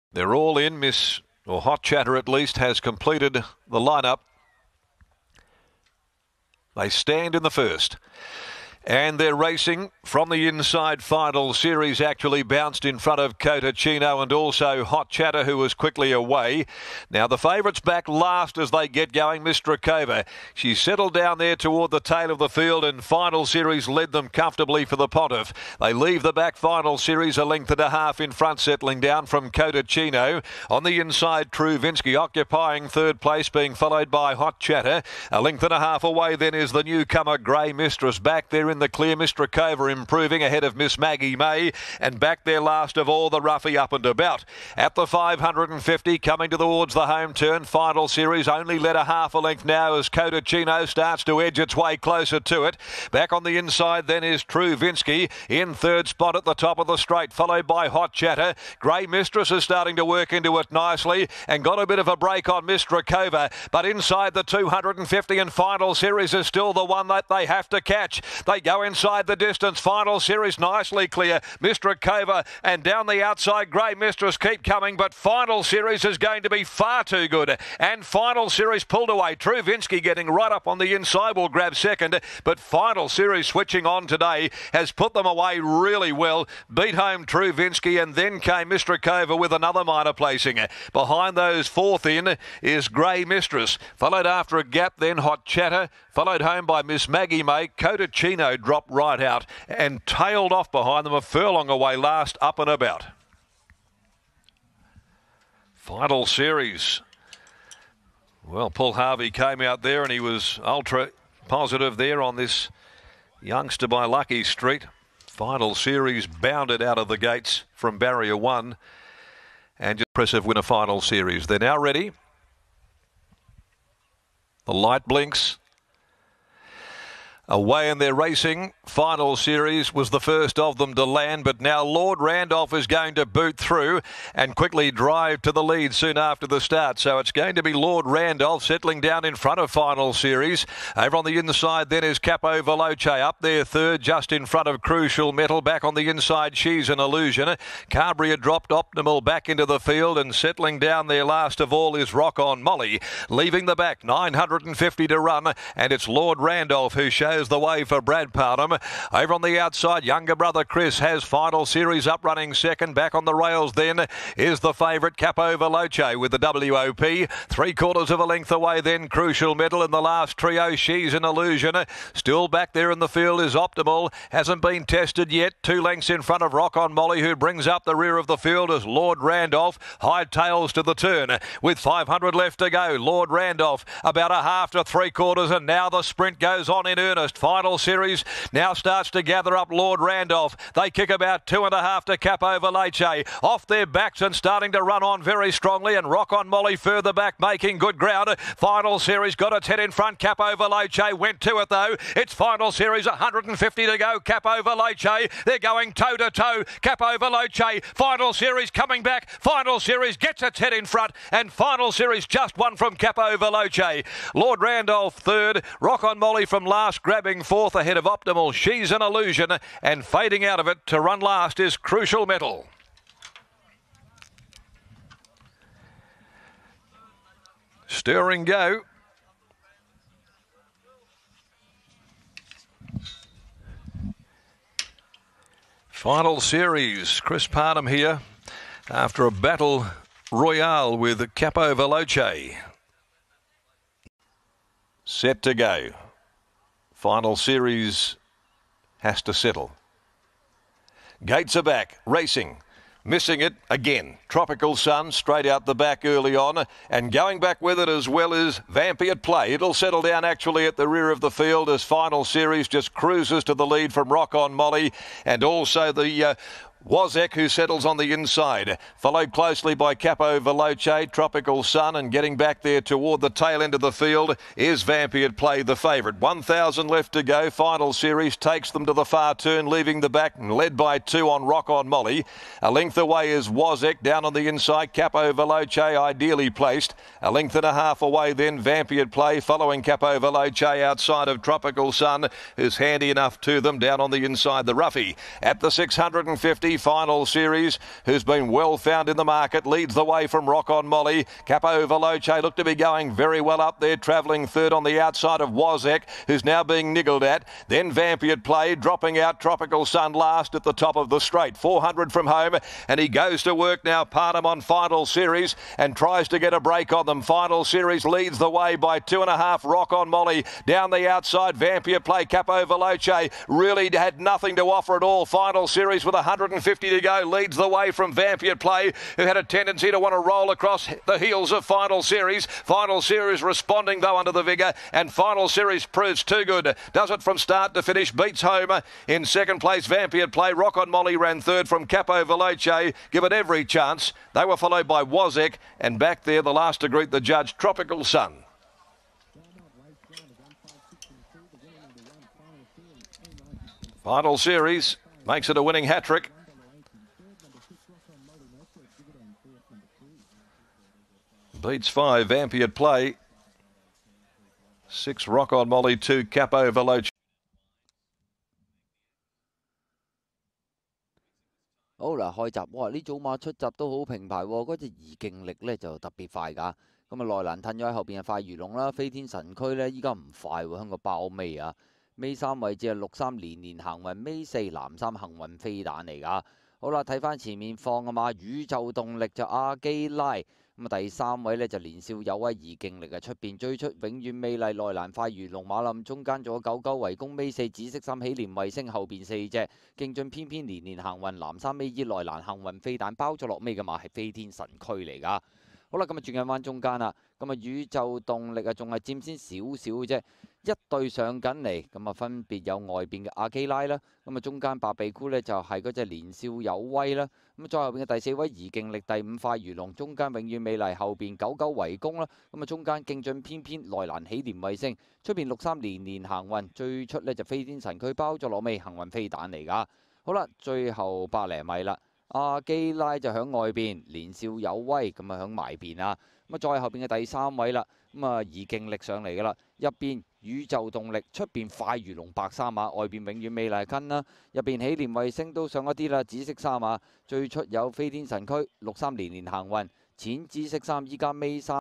【香港賽馬】西澳新星「怡勁力」唔怕你「永遠美麗」，「怡勁力」當地贏馬片重溫，姿態令當地馬迷O晒咀
永遠美麗
怡勁力